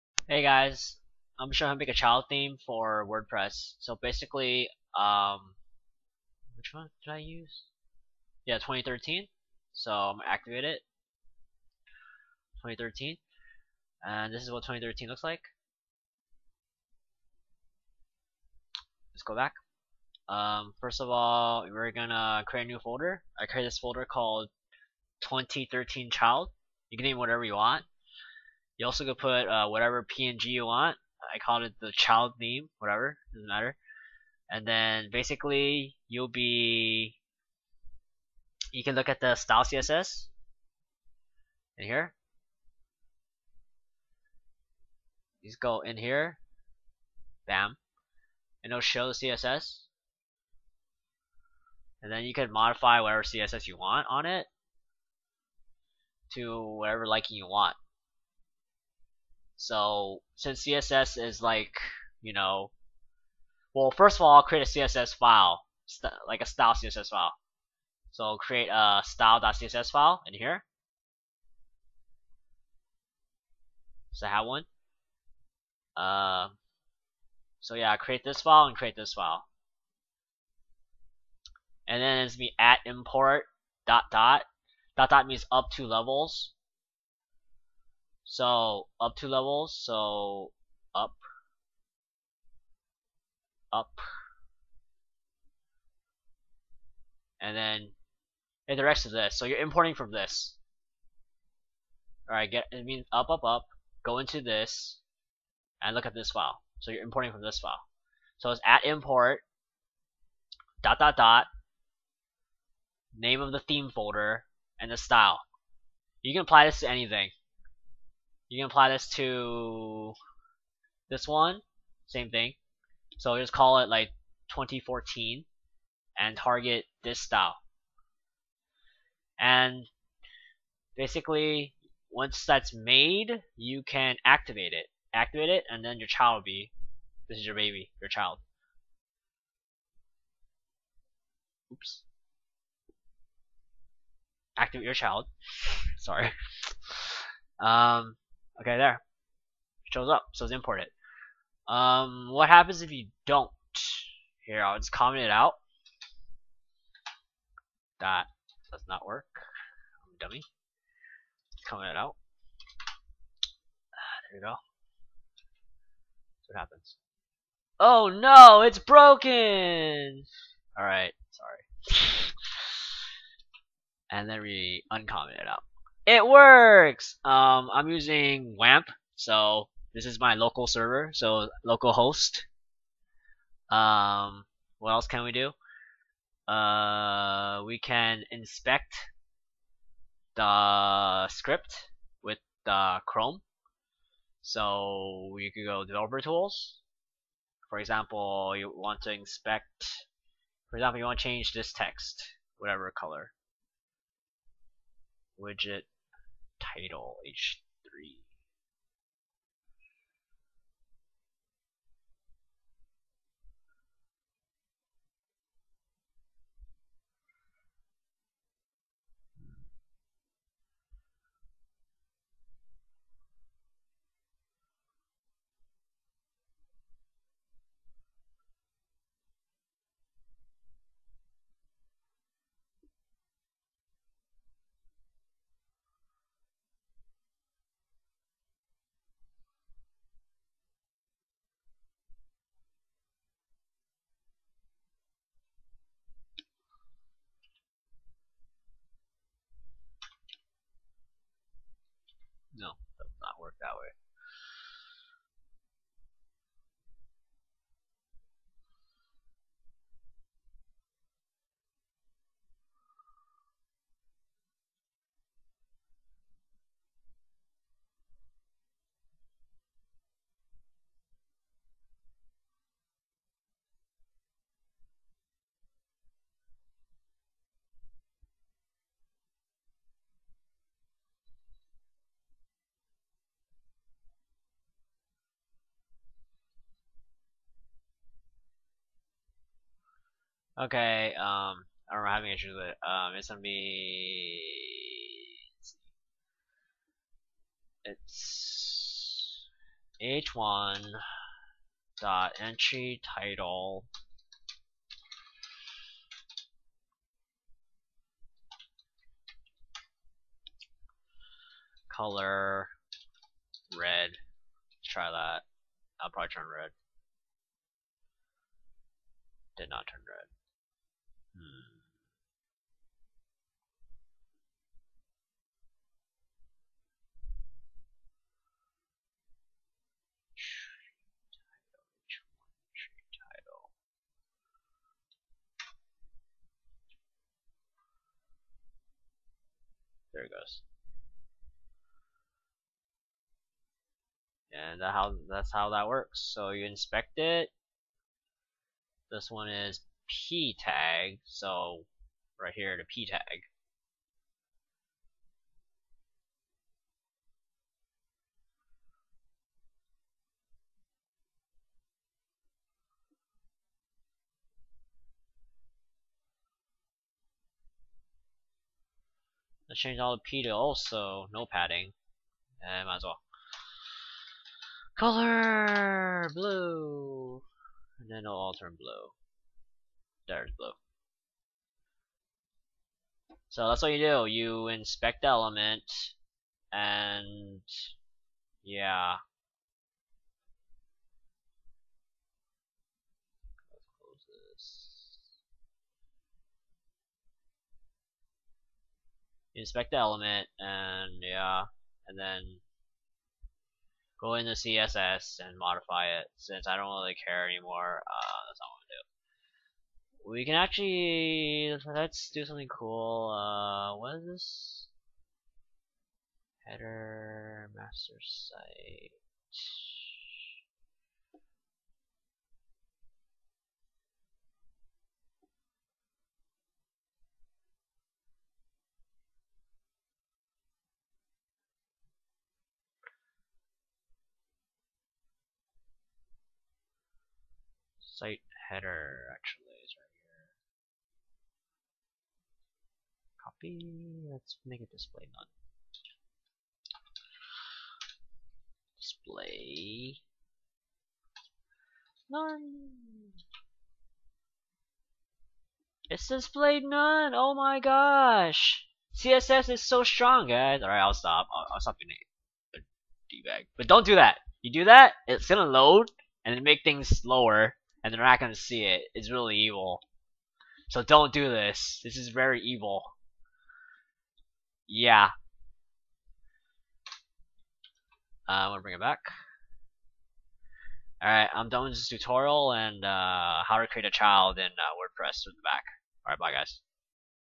Wordpress how to customize your theme via css Child Theme
Wordpress child theming.

We go over how to use the @import "../twentythirteen/style.css";
create style.css pointing to the parent theme css file.
We also use the google chrome dev tools.

upack in this folder:
C:\wamp\www\wordpress\wp-content\themes